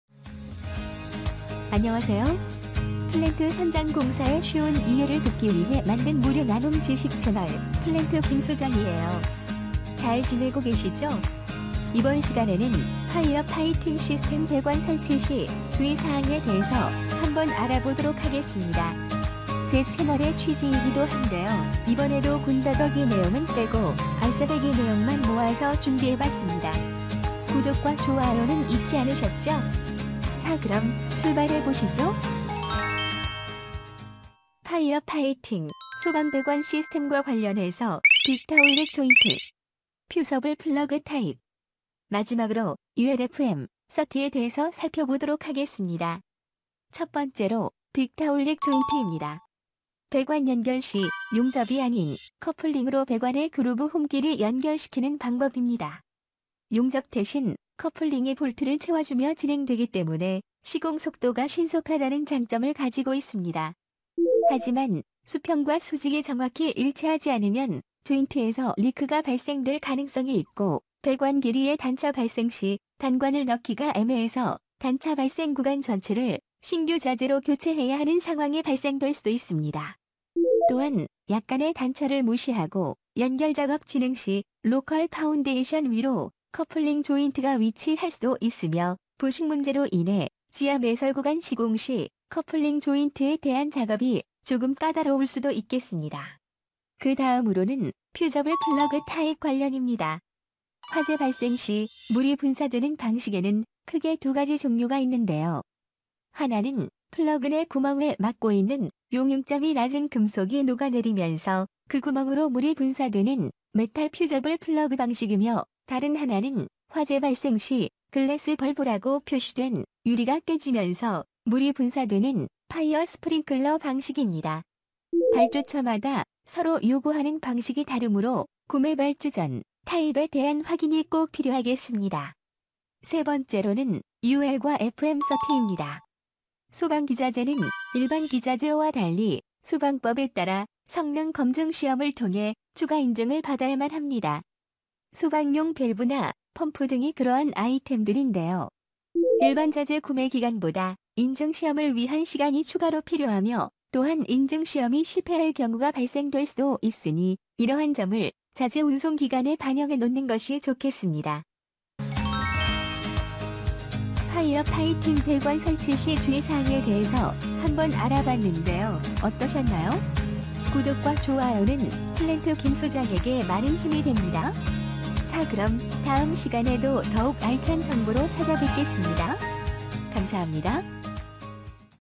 🔥 [플랜트교육] 소방배관 설치하다 사고난다?! 비슷해 보여도 치명적 실수🔥 Victaulic·스프링클러·퓨즈플러그 핵심정리!
✔ 소방배관 설계·시공에 참여하는 플랜트 기술자
✔ Victaulic 조인트 사용 경험이 있는 배관 엔지니어
✔ 스프링클러 헤드 종류 및 기능이 헷갈리는 시공 실무자
✔ FM/UL 인증의 의미가 궁금한 초급 설계 담당자
✔ 플랜트 NCS/자격증 시험 준비 중인 예비 기술인

✅ 소방배관, 아무거나 시공하면 사고로 직행!
현장에서 꼭 알아야 할 Victaulic 조인트, 퓨즈플러그, 스프링클러 헤드, 그리고 UL·FM 인증까지 4분만에 핵심 정리해드립니다.

🔧 **Victaulic 조인트(그루브 조인트)**는 빠른 시공과 우수한 유지보수성을 자랑하지만,
진동·고온 배관에 무분별하게 적용할 경우 오히려 위험 요소가 될 수 있습니다.

🔥 퓨즈플러그는 고온에서 녹는 합금이 내부에 내장되어 있어, 온도 과상승 시 자동 개방되어 장비 보호 역할을 하며, 스팀라인·압축공기·탱크류에 적용됩니다.

🚿 스프링클러 헤드는 종류가 다양합니다.

펜던트형: 천장형 기본 타입
업라이트형: 배관이 바닥보다 낮은 천장 없는 구조물에 사용
사이드월형: 벽면 고정형으로 좁은 공간에 효과적
감지방식: 글래스벌브 or 융제합금(퓨즈)

💡 모든 스프링클러는 화재 열기 감지 → 해당 위치만 자동 방수,
최소한의 피해로 최대의 효과를 내는 시스템입니다.

🔒 UL과 FM 인증은 단순한 인증이 아닙니다.

UL (미국 보험업자 실험소): 세계적으로 통용되는 안전 인증 기준
FM (Factory Mutual): 화재 손실 예방을 위한 설계 및 성능 기준
두 인증이 동시에 적용된 부품은 국제적으로 인정되는 신뢰의 상징입니다.